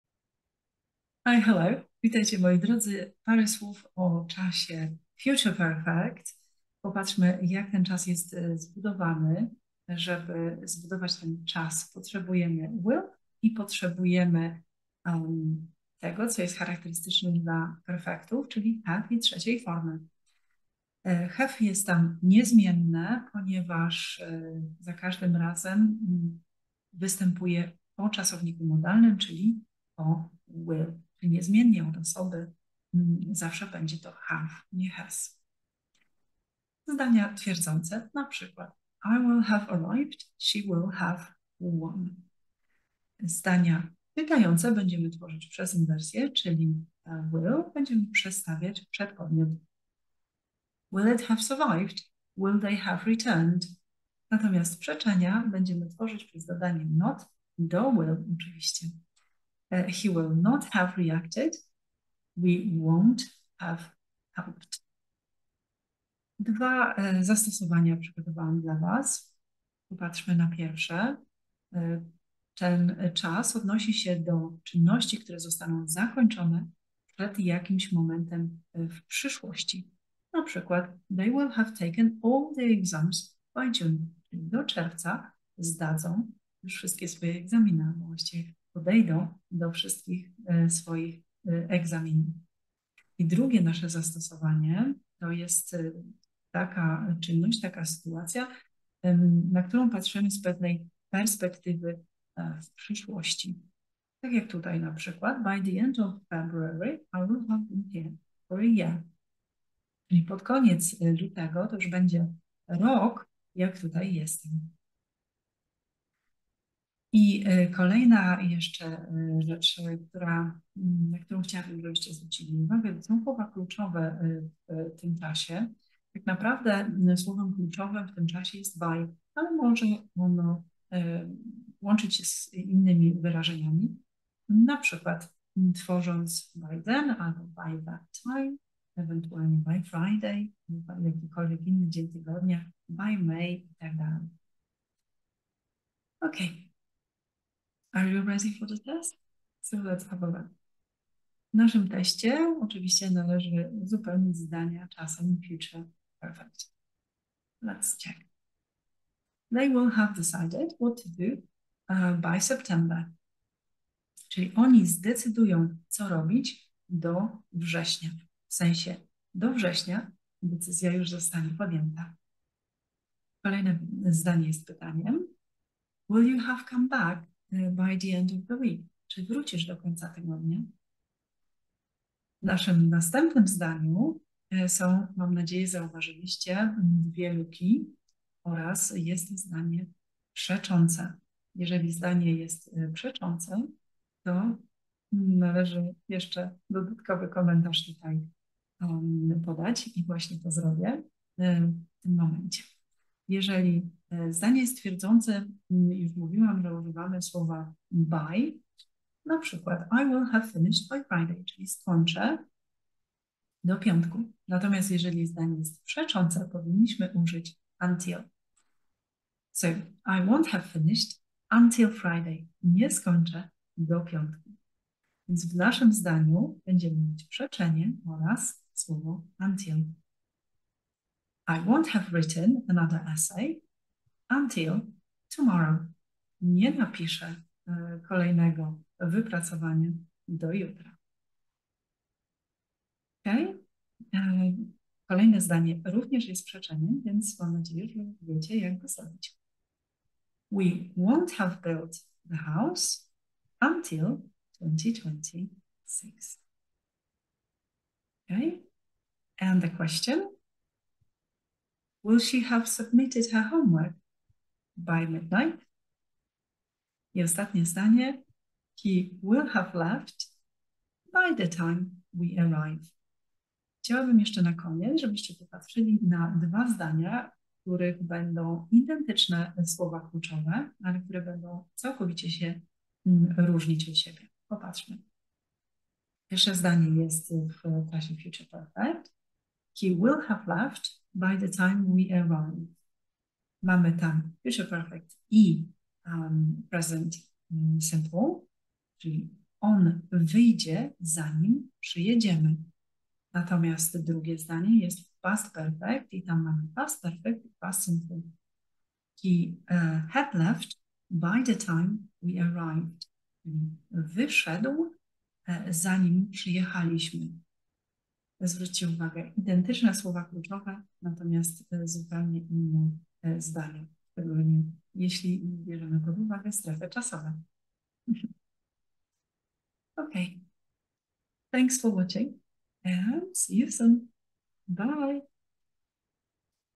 TEST - czas Future Perfect - budowa, zastosowanie + test z dodatkowym komentarzem.
Jak zbudować czas Future Perfect? Kiedy stosujemy czas Future Perfect? Krótkie wyjaśnienie wraz z testem na zakończenie.